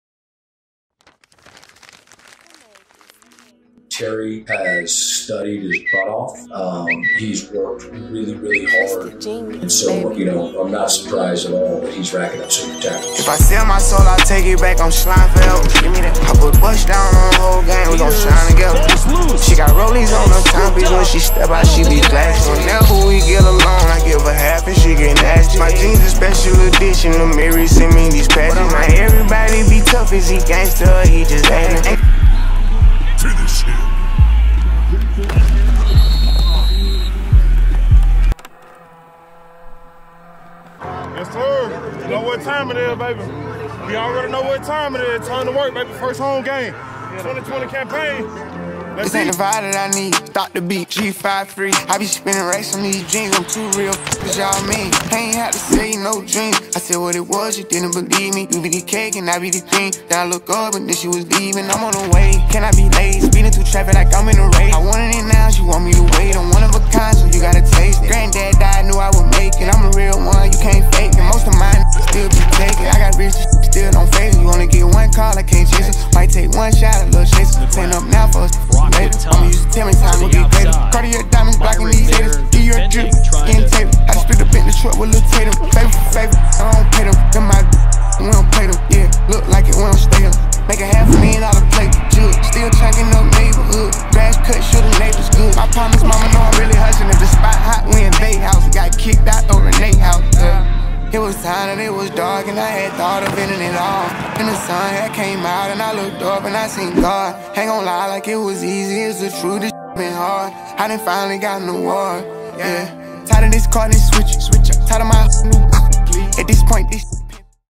Terry Myrick 2020 COVID CAMPAIGN
Terry Myrick | Eastern Michigan Football
2020 6 Game Highlight Reel
2nd Team All-MAC